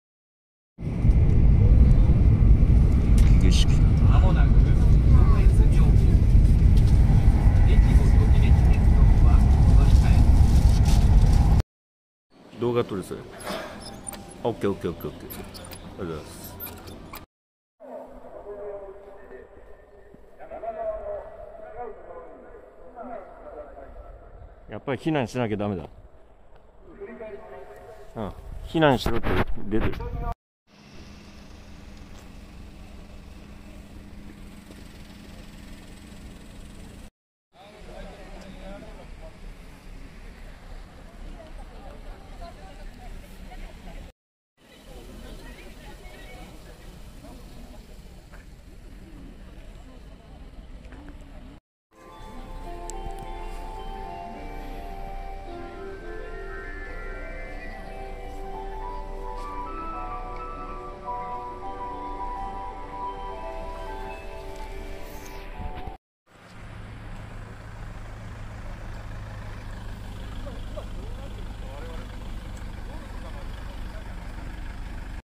能登半島地震 直後の地元映像〜新潟県糸魚川市（2024/01/01）〜
・2023年大晦日、
実家に帰省していた。

翌日の2024年、
家族で日帰り温泉に出掛けた。

帰り道の車内で、
緊急地震速報が鳴る。

能登半島で大地震が
起きてることを知る。

実家のほうで、
急いで避難場所に逃げる。

妹なんかは、通帳なども持って
クルマで高台まで逃げた。

私と弟、母は近くの神社が
避難場所となっており、

そこに避難したのだった。

とはいえ、海も近いし、
津波の不安も拭いきれなかった…。